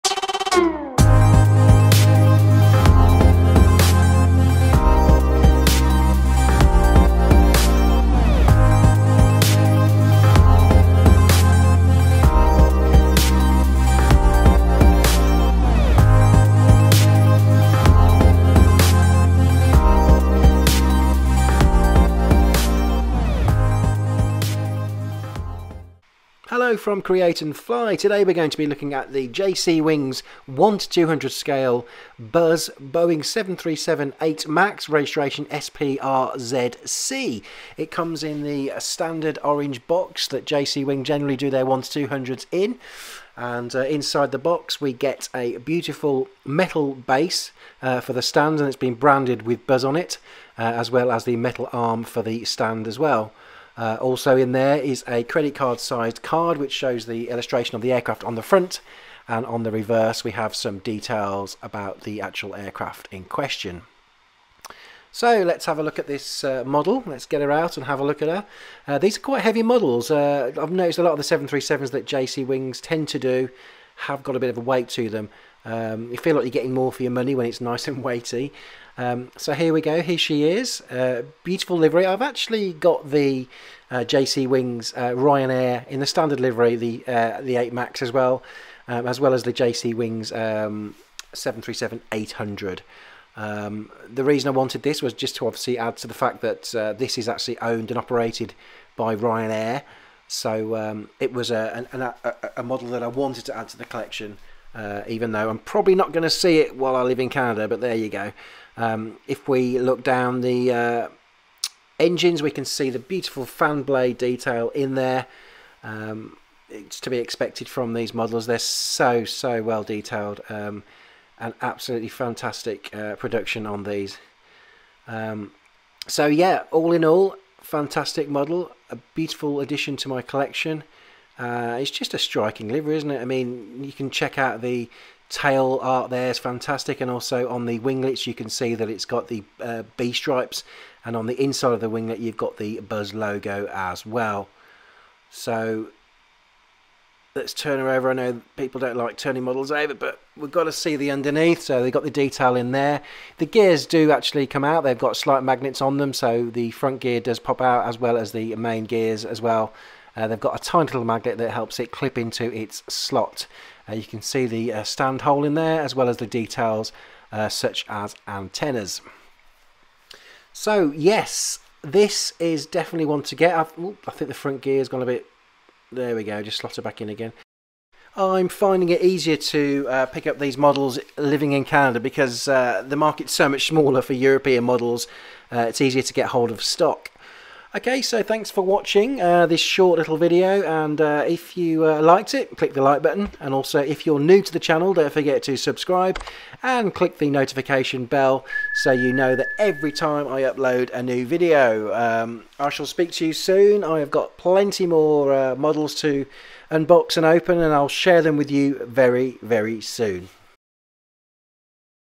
JC Wings Buzz Airlines / Ryanair Boeing 737 MAX 8. 1/200 Scale. Quick Overview.
Here I show off my JC Wings 1/200 scale model of the Boeing 737 MAX 8 in Buzz Airlines livery.

Stunning, heavy-weighted model with all the details you'd expect from these JC Wings releases.

I own the copyright to the music. Recorded and produced by Nathan Bennett 2023.